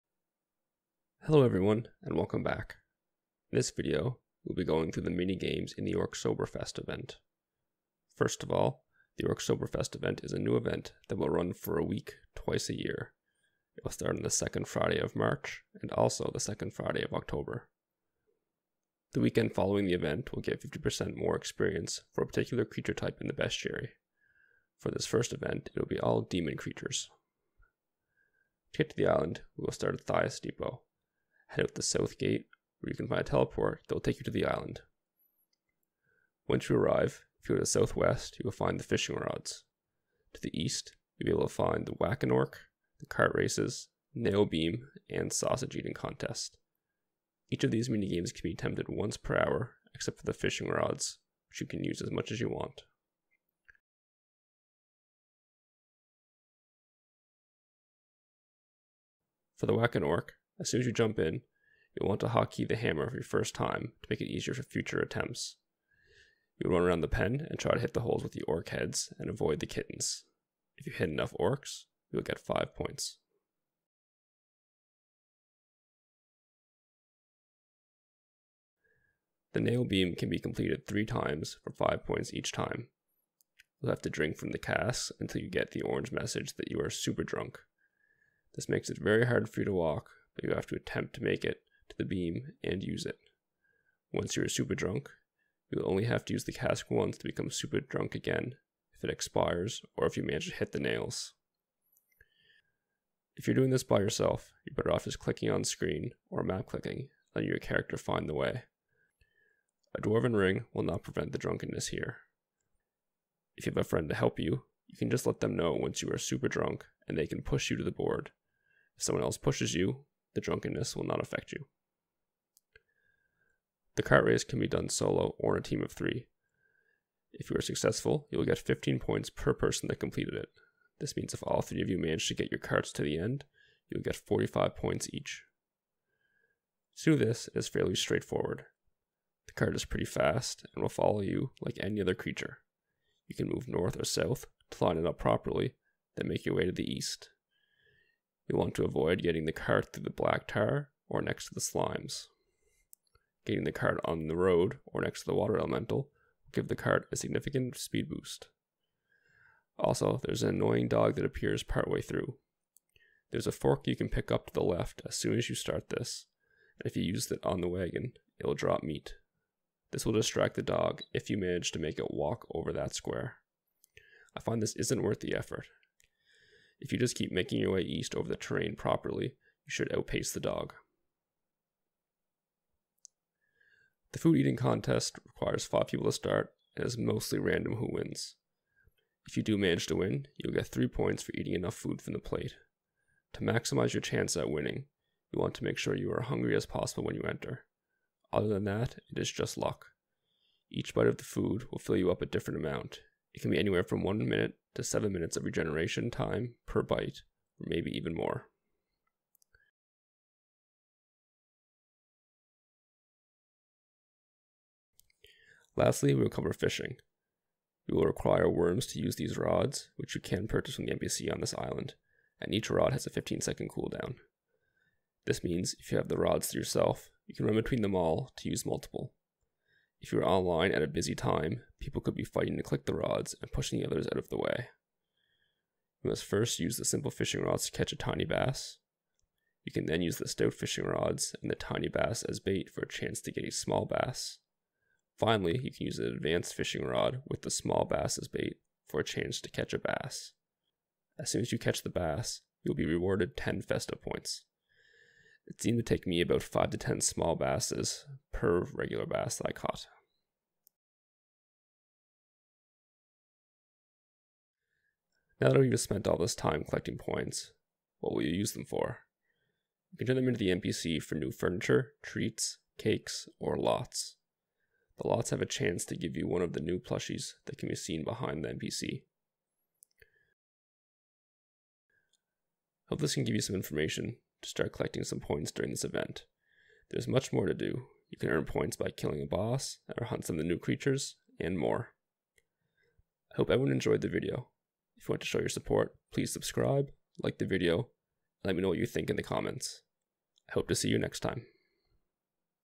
Orcsoberfest - Mini Games
In this video we are discussing the new Orcsoberfest event, where to find the new island, and how to complete the mini games.